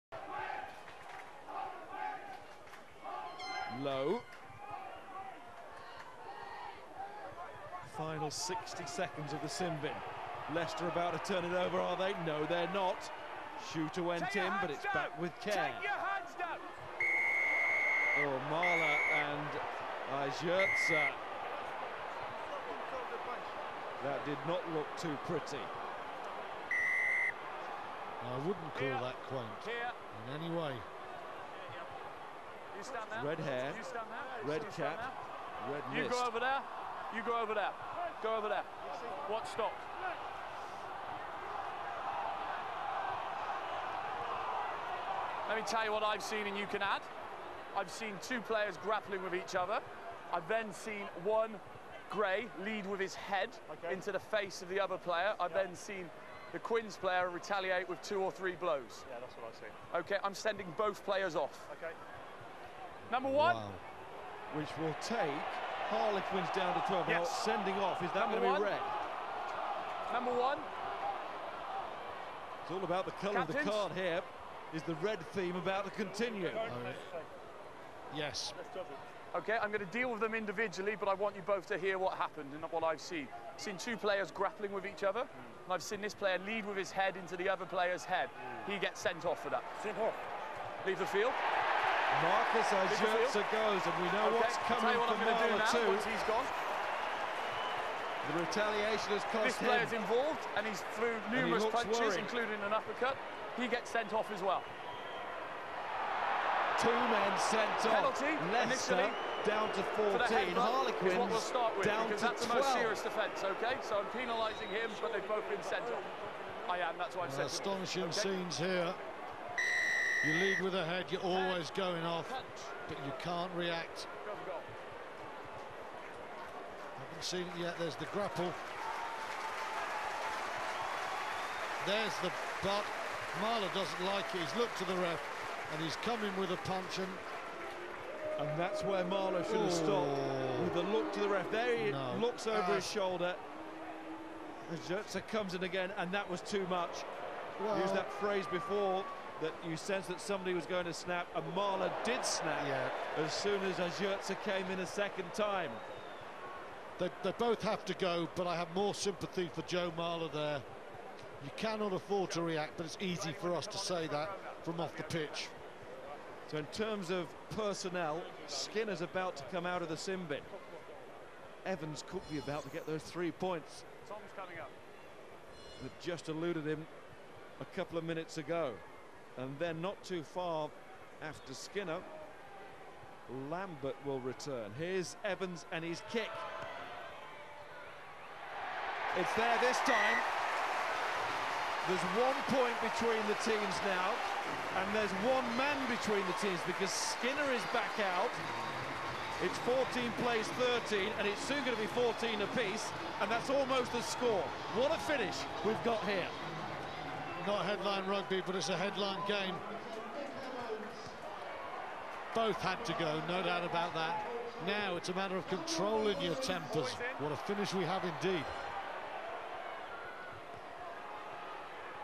Joe Marler and Marcos Ayerza fight
Joe Marler of Harlequins and Marcos Ayerza of Leicester Tigers have a fight in the Aviva Premiership match and both get sent off.